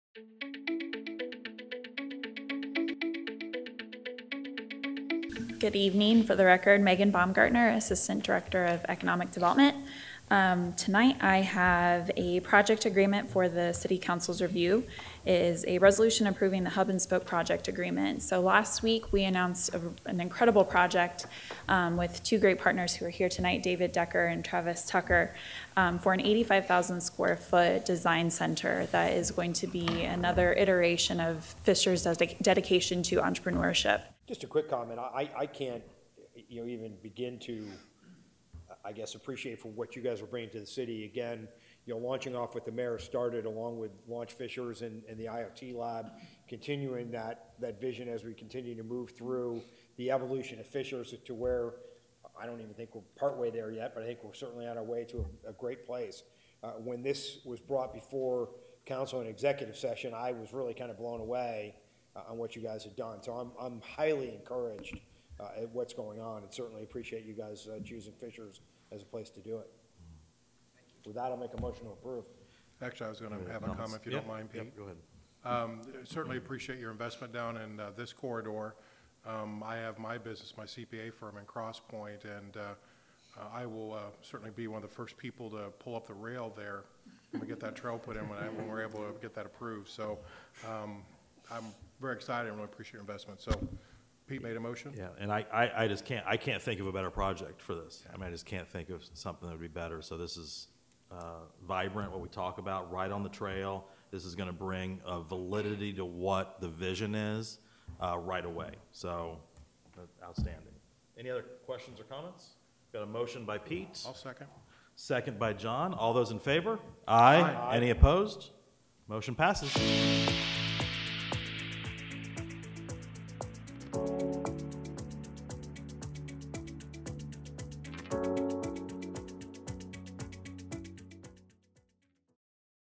Council Approval of Hub & Spoke
Fishers City Council approving the Hub & Spoke project coming to Fishers Nickel Plate District this year!